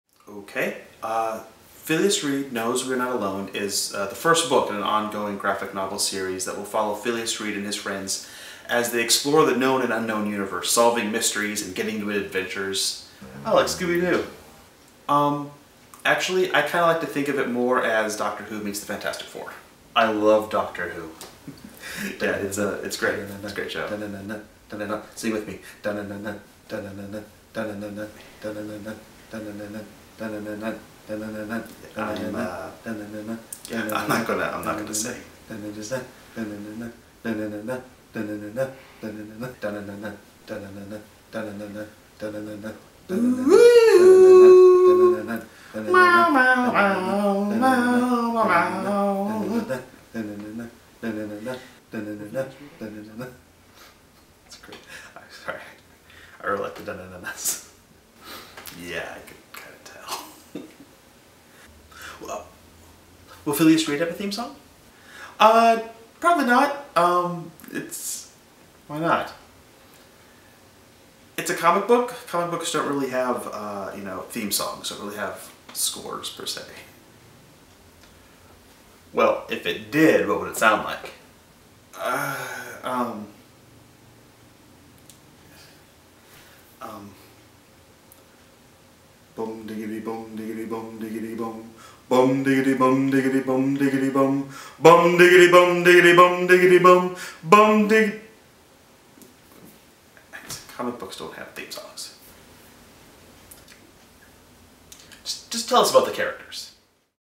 Doctor Who Deleted Scene (from our Kickstarter)
Got a little carried away with myself during the making of our Kickstarter video and broke out into a duet with myself.

You can see the video this was cut from here (until December 1st)